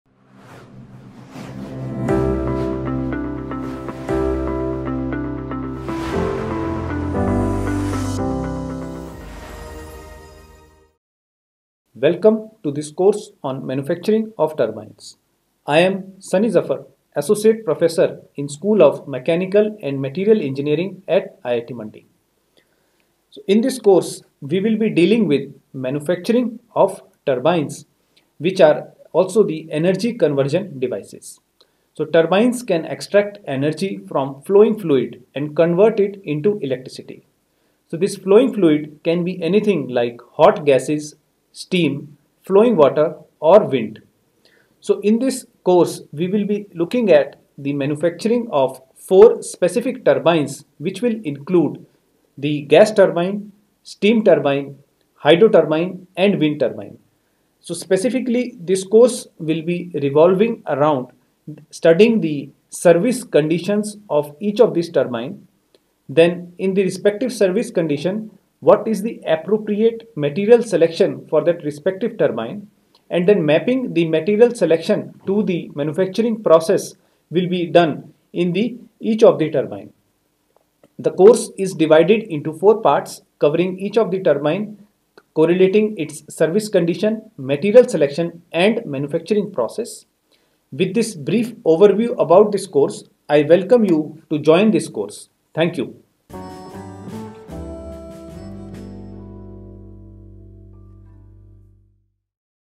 Introduction_Manufacturing of Turbines (Gas, Steam, Hydro and Wind)
Prof. Sunny ZafarSchool of Mechanical and Materials EngineeringIndian Institute of Technology Mandi